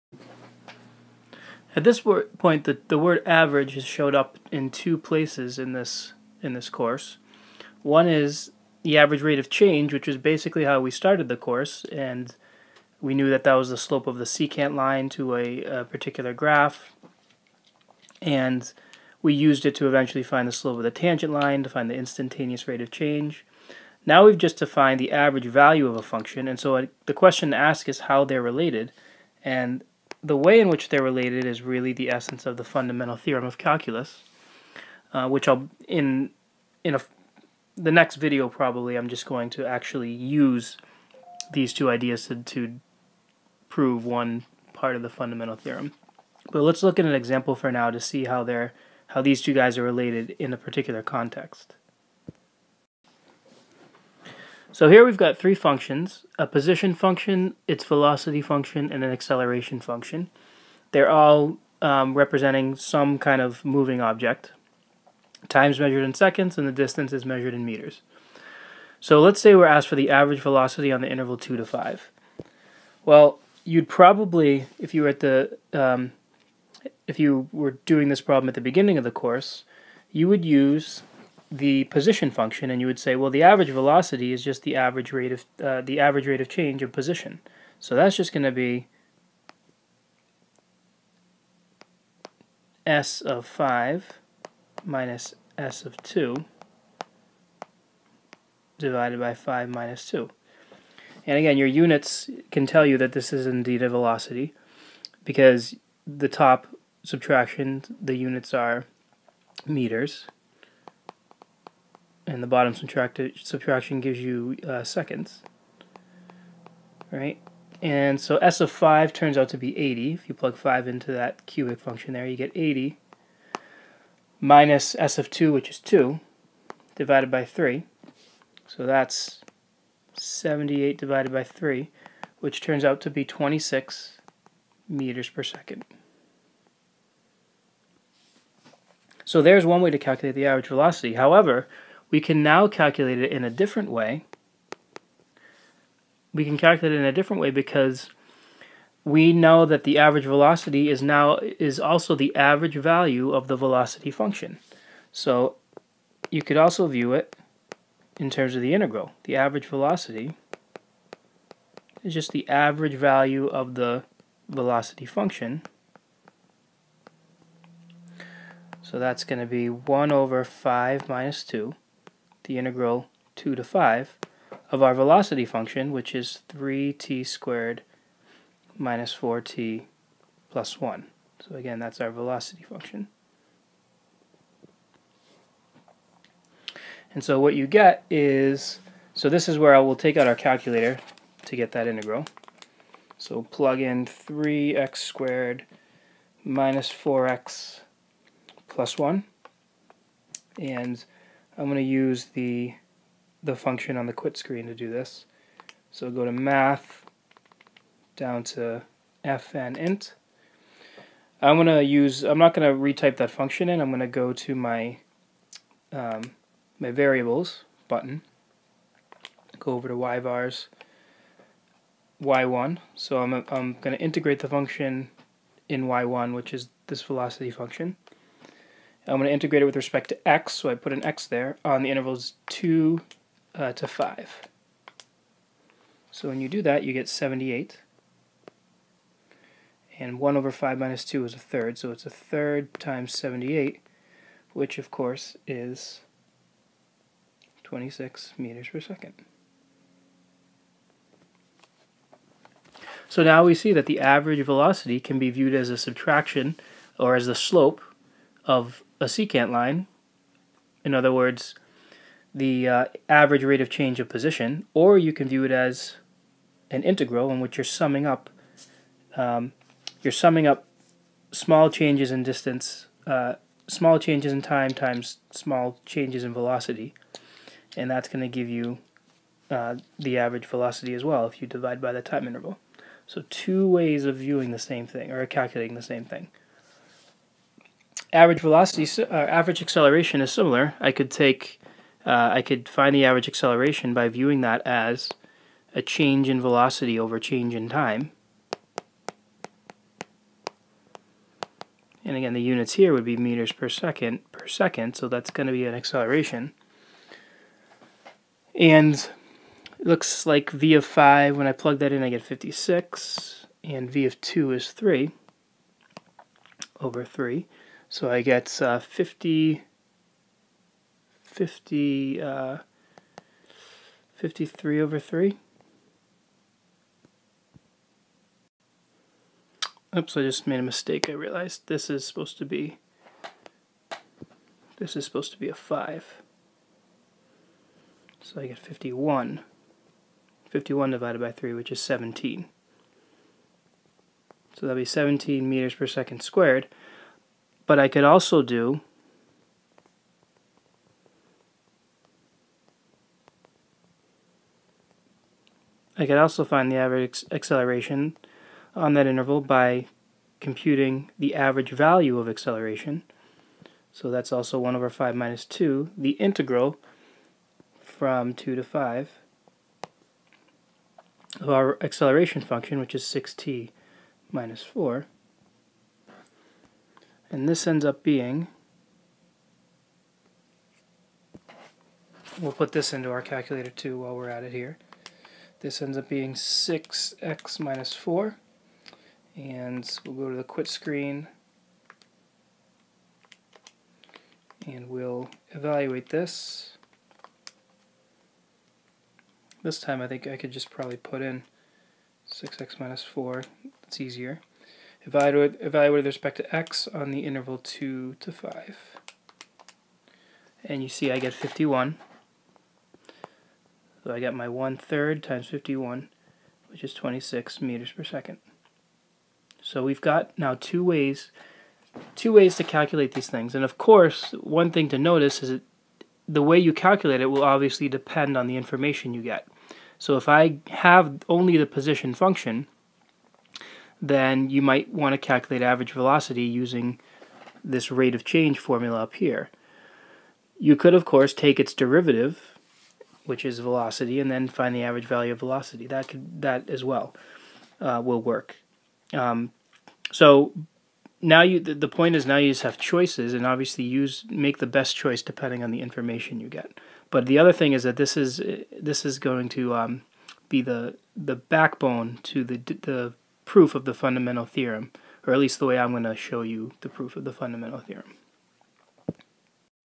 Calculating Average Velocity in Two Ways
Calculating the average velocity from the position function as a rate of change and from the velocity function as an average value